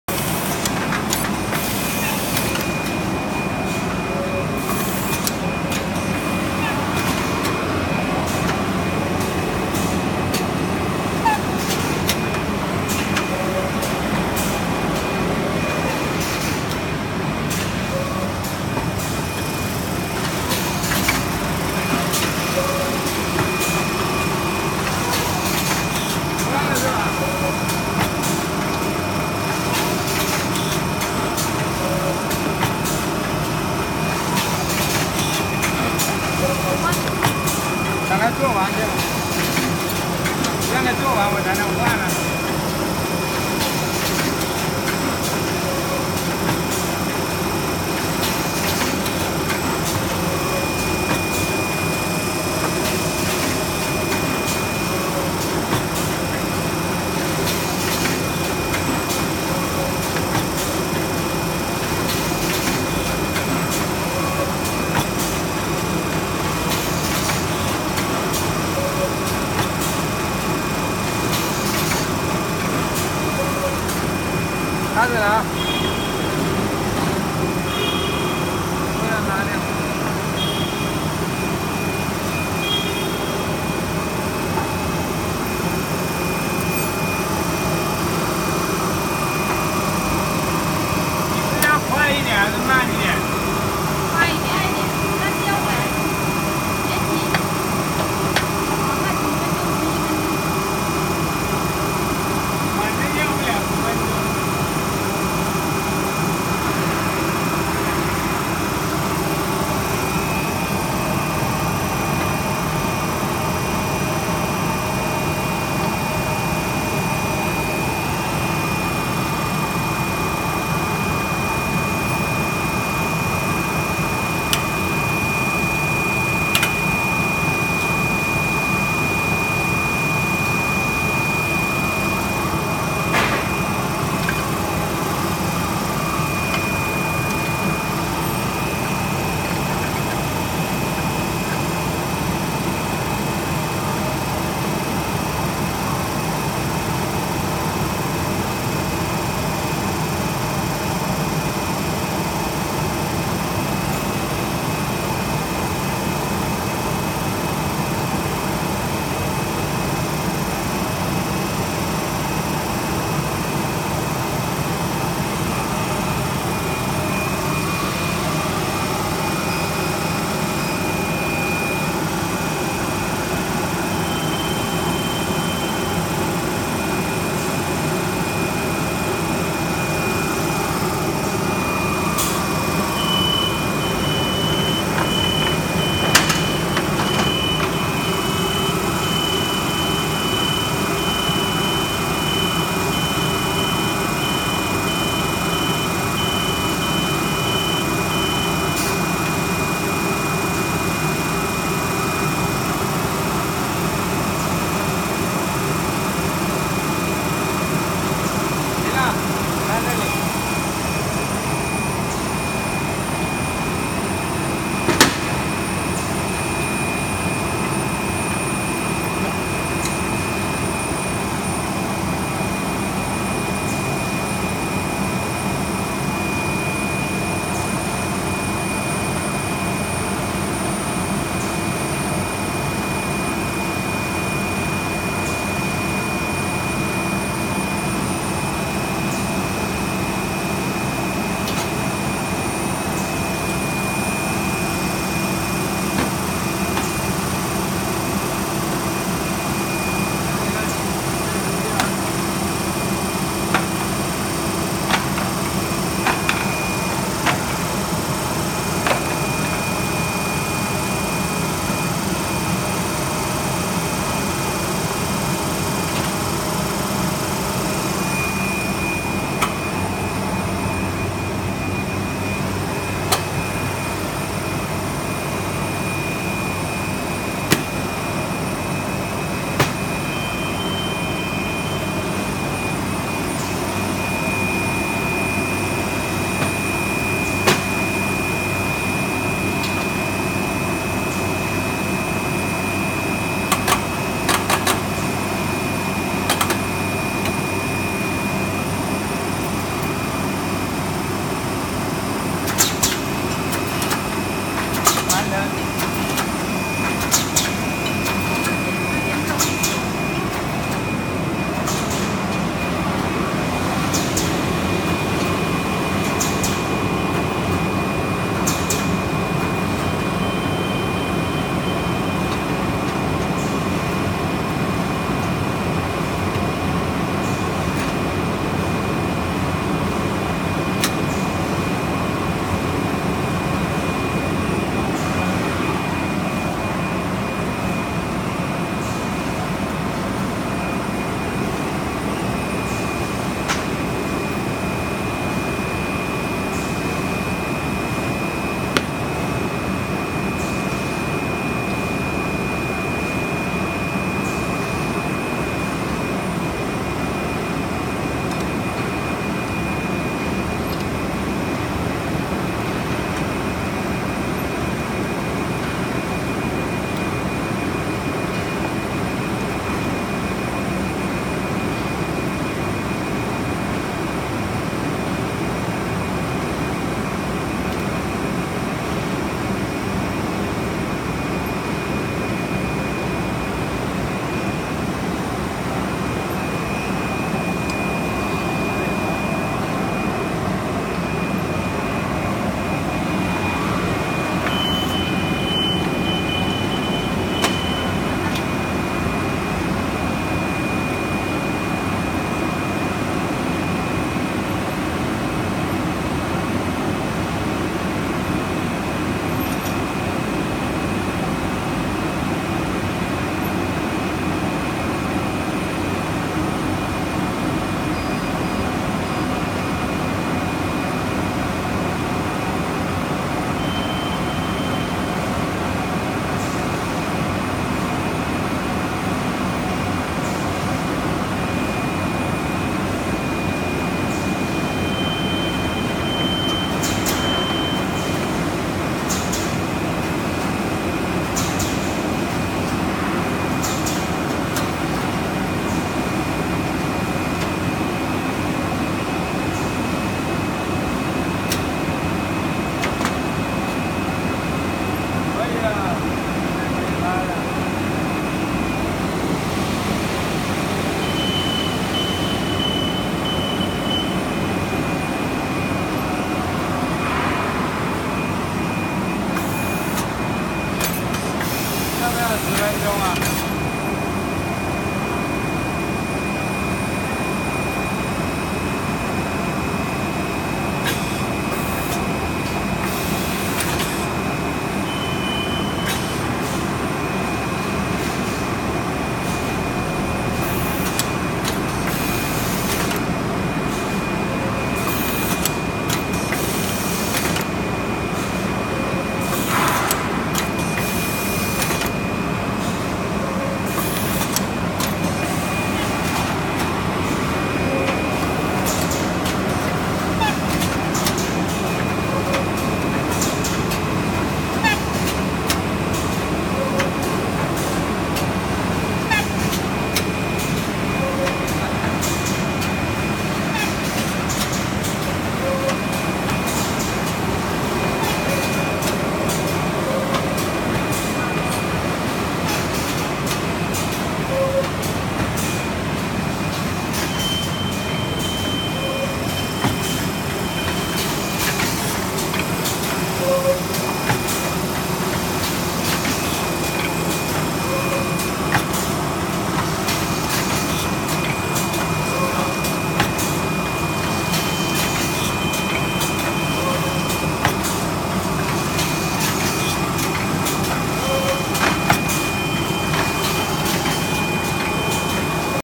HM-ZD350K Smart rigid box machine ( size change within 10 mins) skype yvettewang18
HM-ZD350K Smart rigid box machine ( size change within 10 mins) skype yvettewang18 . in this video we are showing how to change the size on HMZD 350K, this machine suitable for small quantity production, fast and stable. email mobile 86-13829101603